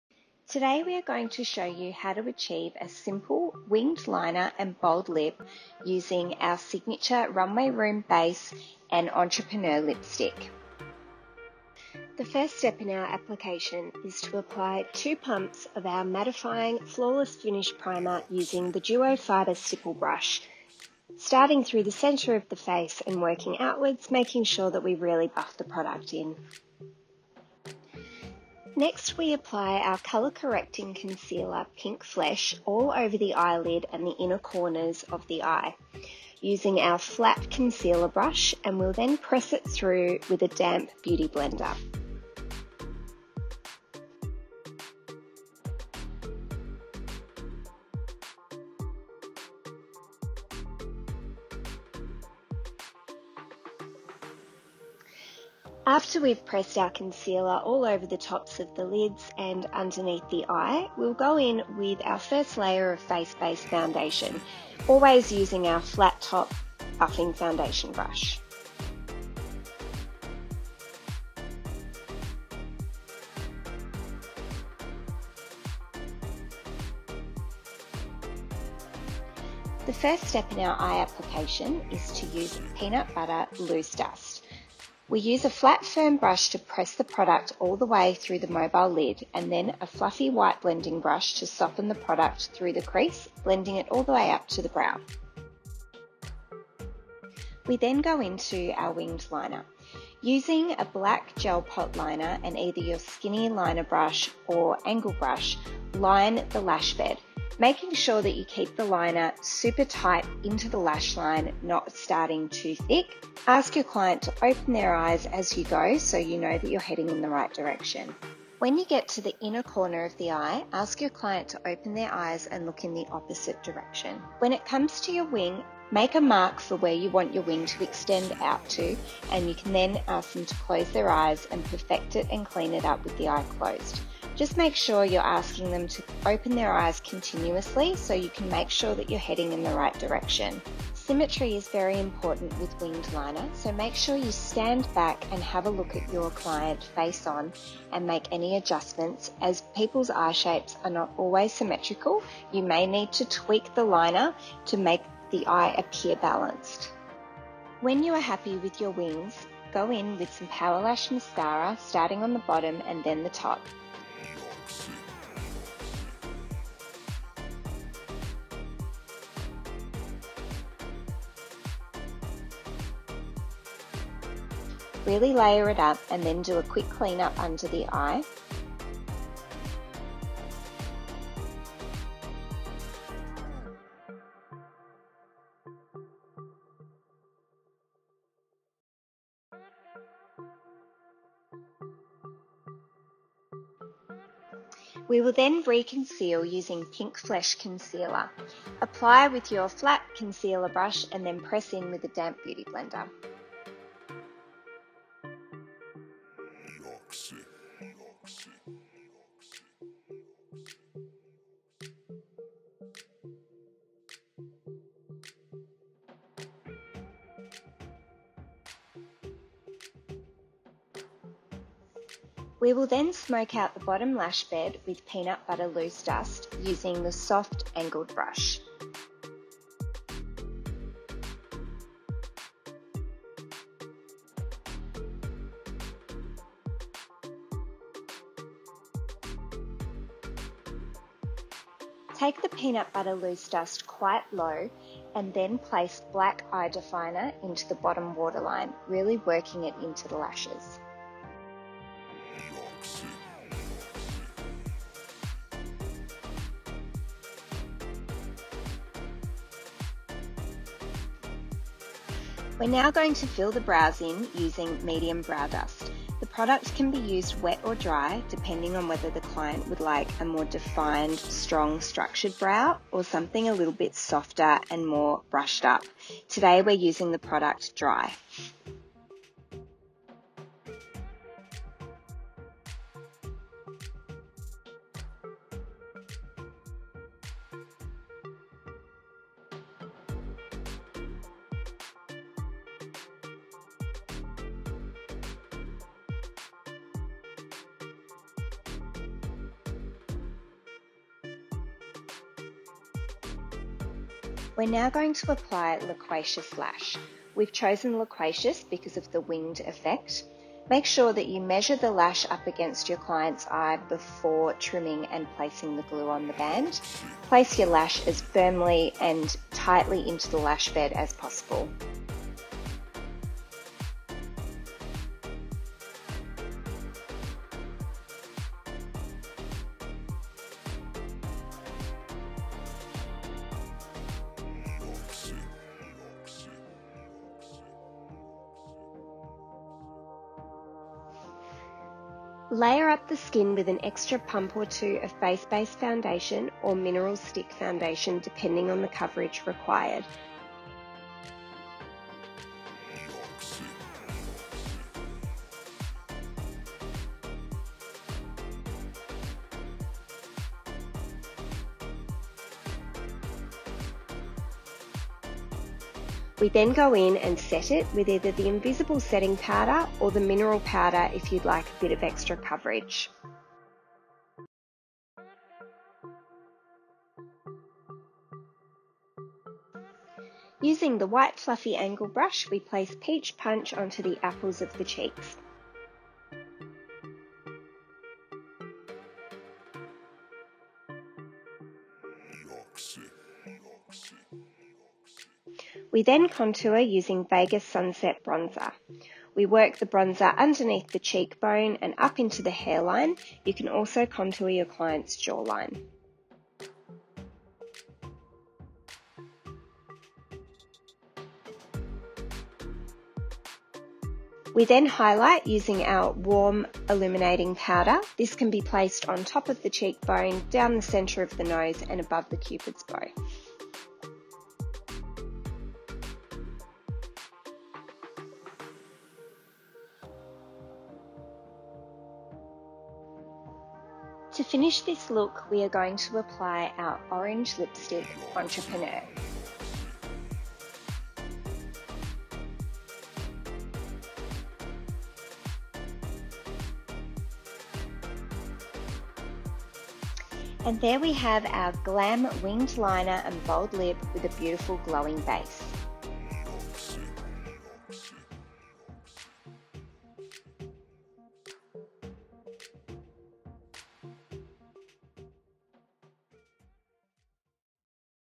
Winged Liner and Bold Lip Tutorial on Mia Fevola | RUNWAY ROOM
Head makeup artist, Jess, talks us through how to achieve this stunning glam featuring a bold lip and a winged liner.